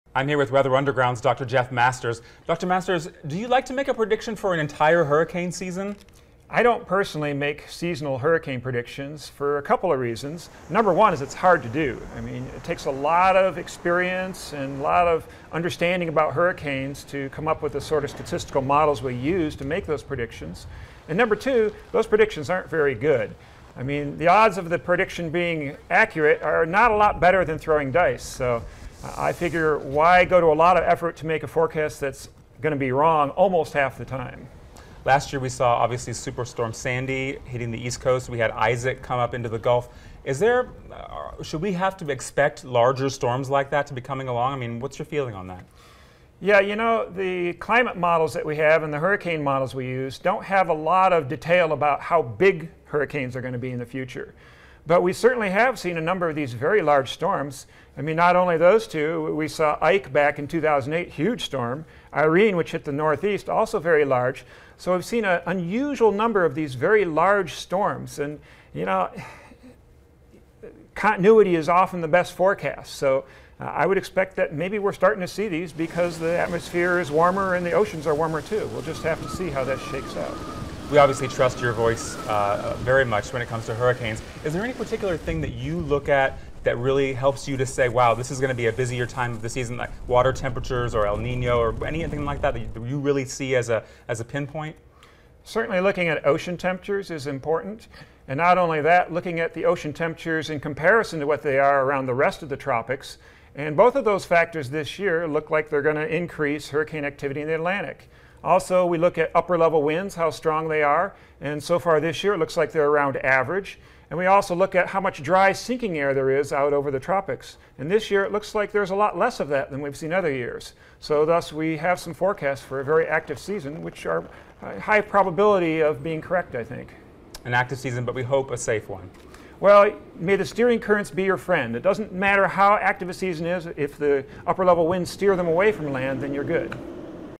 Is It Irresponsible to Predict Entire Hurricane Season?
Dr. Jeff Masters with Weather Undergound talks about forecasting the hurricane season and the difficulties involved.